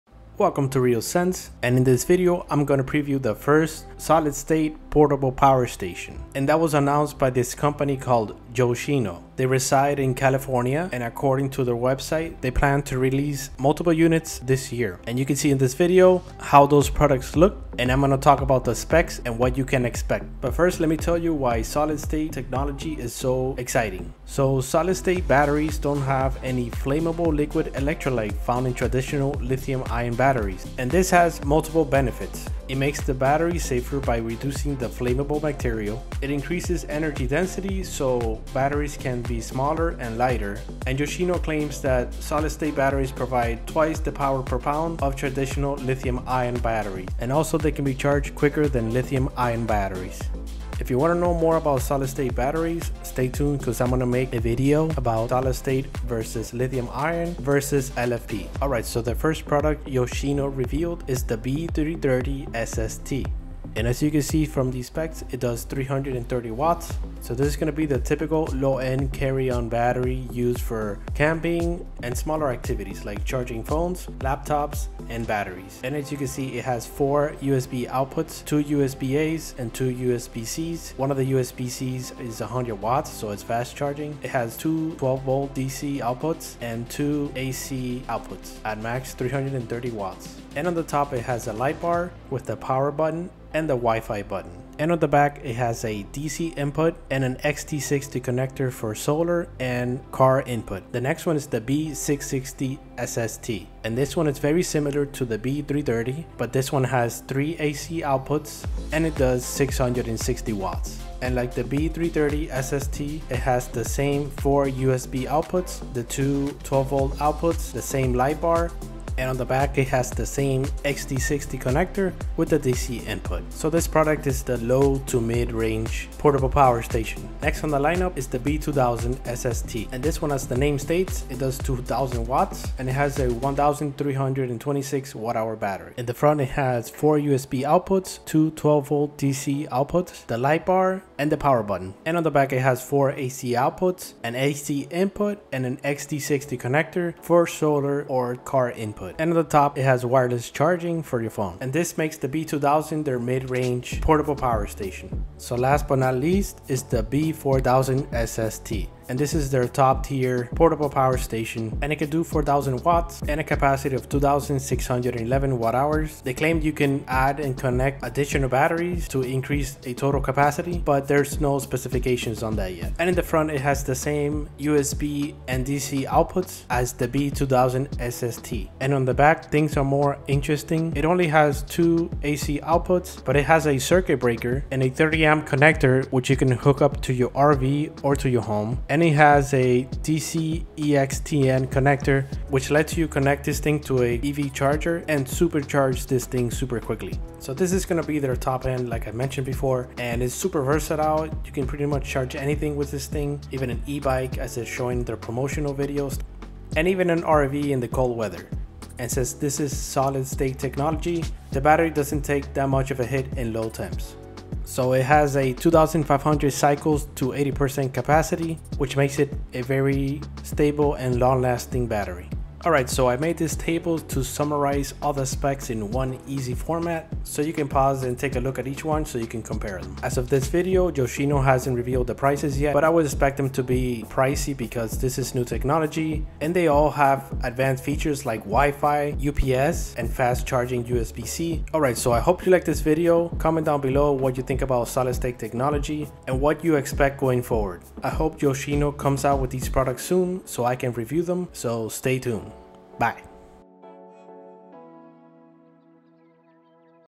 Yoshino Solid State Portable Power Stations
In this video I preview the exciting new Solid State Portable Power Stations revealed by Yoshino. They are next gen batteries with new and improved safety, energy density, and fast charging capabilities. For more information you can check out the Yoshino